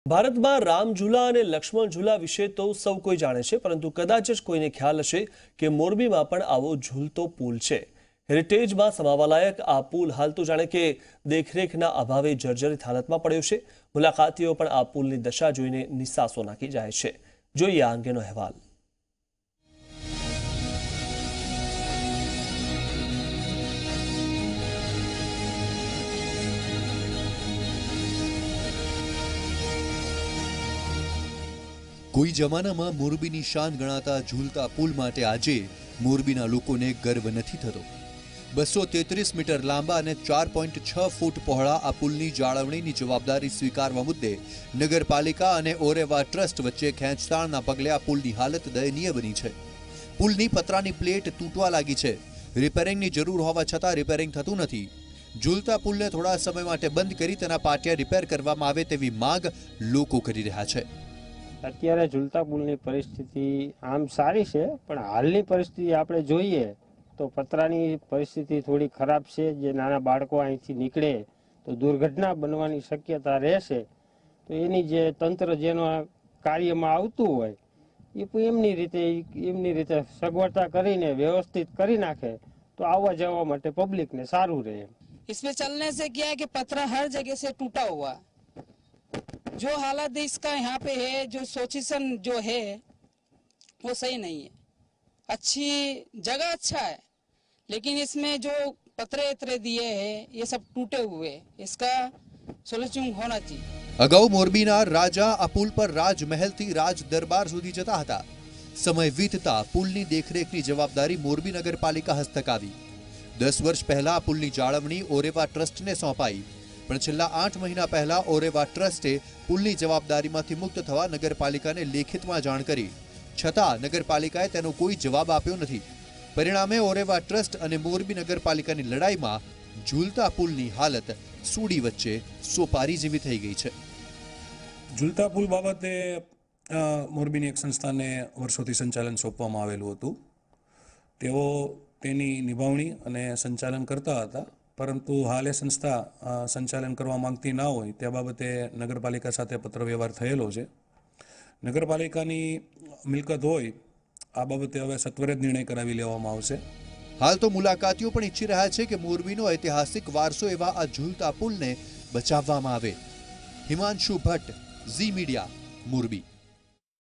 What is reality of Morbi's famous suspension bridge? - Zee 24 Kalak
What is reality of Morbi's  famous suspension bridge?